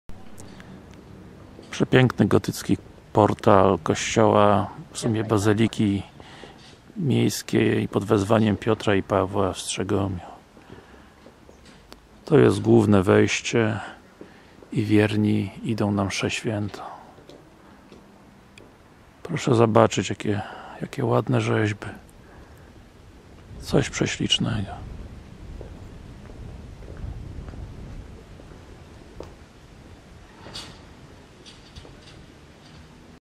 Strzegom: Portal Bazyliki Mniejszej
Naprawdę ciekawe wejście do tego kościoła, które każdy turysta powinien zobaczyć